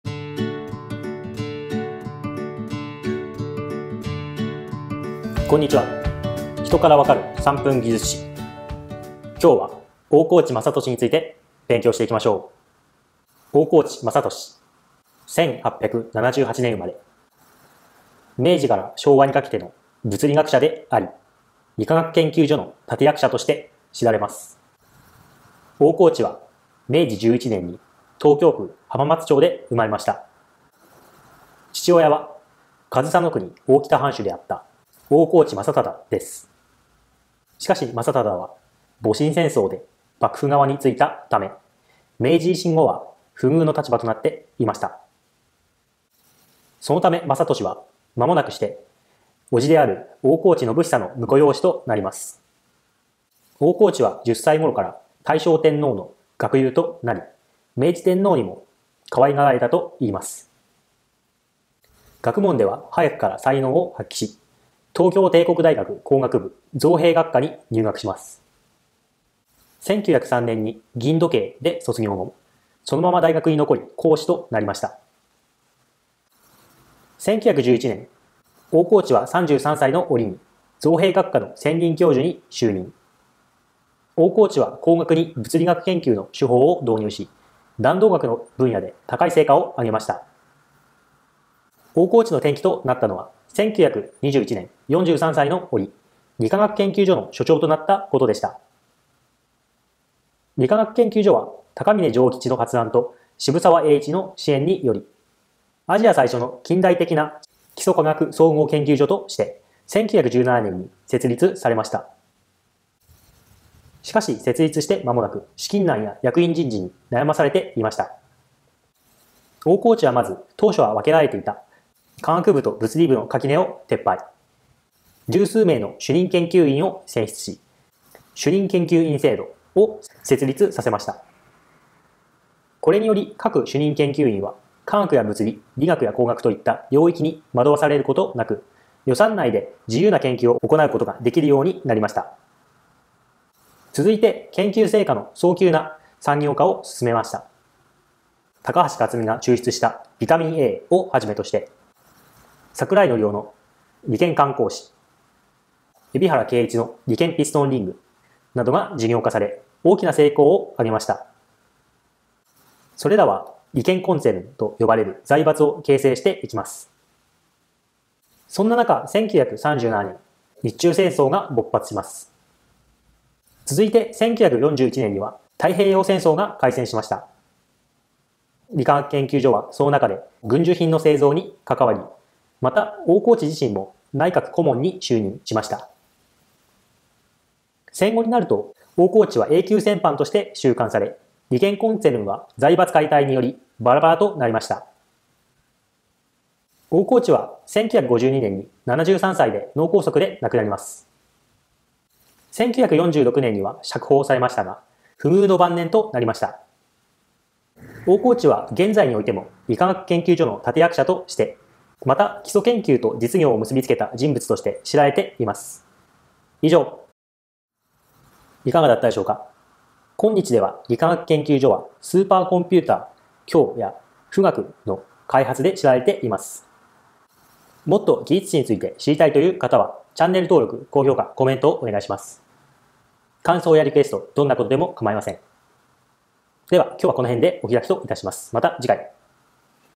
3分でわかる大河内正敏【理化学研究所の立役者】
明治から昭和にかけての物理学者であり、理化学研究所の立役者として知られる大河内正敏を紹介します。

こんにちは。人からわかる3分技術史。
今日は大河内正敏について勉強していきましょう。

大河内正敏。1878年生まれ。明治から昭和にかけての物理学者であり、理化学研究所の立役者として知られます。

大河内は、明治11年に東京府浜松町で生まれました。
父親は上総国大多喜藩主であった大河内正質です。しかし正質は戊辰戦争で幕府側に着いたため、明治維新後は不遇の立場となっていました。そのため正敏は、間もなくして伯父である大河内信古の婿養子となります。
大河内は10歳ごろから大正天皇の学友となり、明治天皇にも可愛がられたといいます。
学問では早くから才能を発揮し、東京帝国大学工学部造兵学科に入学します。1903年に銀時計で卒業後、そのまま大学に残り講師となりました。
1911年、大河内は33歳のおりに造兵学科の専任教授に就任。大河内は工学に物理学研究の手法を導入し、弾道学の分野で高い成果を上げました。
大河内の転機となったのは、1921年、43歳のおり、理化学研究所の所長となったことでした。
理化学研究所は、高峰譲吉の発案と渋沢栄一の支援により、アジア最初の近代的な基礎科学総合研究所として1917年に設立されました。しかし設立して間もなく、資金難や役員人事に悩まされていました。
大河内はまず、当初は分けられていた化学部と物理部の垣根を撤廃。十数名の主任研究員を選出し、「主任研究員制度」を成立させました。
これにより、各主任研究員は化学や物理、理学や工学といった領域に惑わされることなく、予算内で自由な研究を行うことができるようになりました。
続いて研究成果の早急な産業化を進めました。高橋克己が抽出したビタミンAをはじめとして、櫻井季雄の理研感光紙（現・リコー）、海老原敬吉の理研ピストンリング（現・リケン）などが事業化され、大きな成功を遂げました。それらは理研コンツェルンと呼ばれる財閥を形成していきます。
そんななか1937年、日中戦争が勃発します。続いて1941年には太平洋戦争が開戦しました。
理化学研究所はその中で、軍需品の製造に関わり、また大河内自身も内閣顧問に就任しました。
戦後になると、大河内はA級戦犯として収監され、理研コンツェルンは財閥解体によりばらばらとなりました。

大河内は、1952年に73歳で脳梗塞で亡くなります。1946年には釈放されましたが、不遇の晩年となりました。大河内は現在においても、理化学研究所の立役者として、また、基礎研究と実業を結びつけた人物として知られています。

以上！